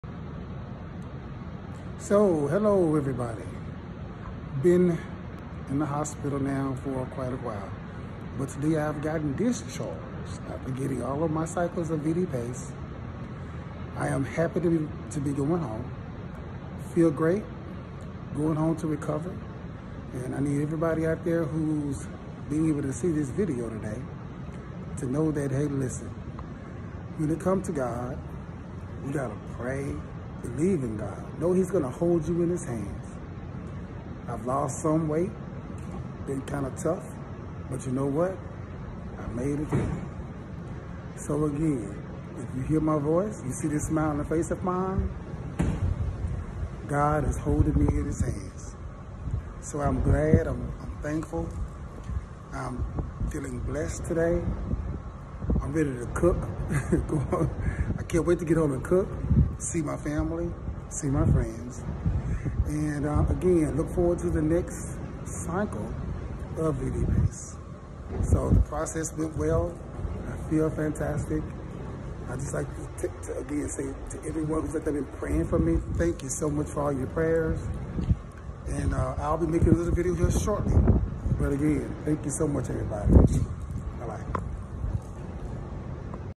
Discharged After VD-PACE And Feeling Good.
Big "D" is Headed Home To See His Family and to Get Some Real Food. I am Not In Any Pain, I am Truly Thankful to god. I Would Like To Thank all YouTube Friends and Supports for all the prays, I feel Blessed on this Special Day..